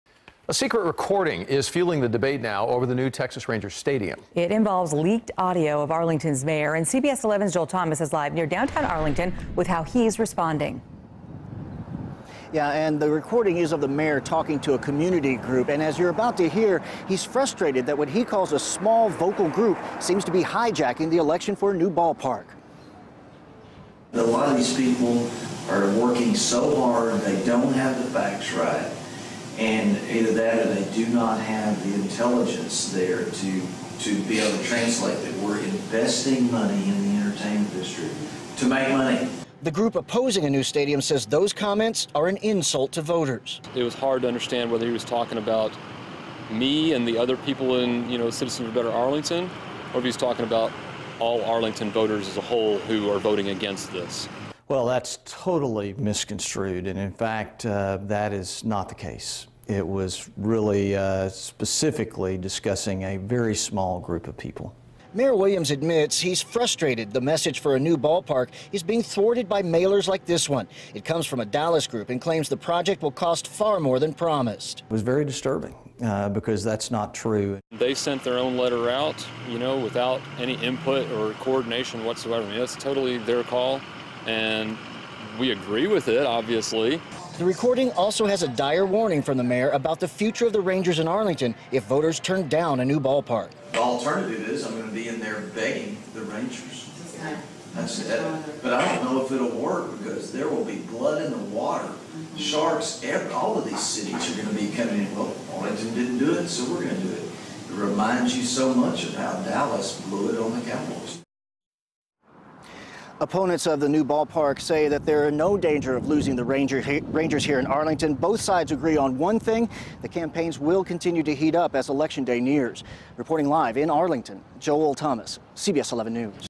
Group Opposing Arlington Ballpark Releases Secretly-Recorded Remarks By Mayor
Opponents of a proposed retractable roof ballpark for the Texas Rangers are taking the mayor of Arlington to task over secretly-recorded remarks made during a meeting with local business leaders.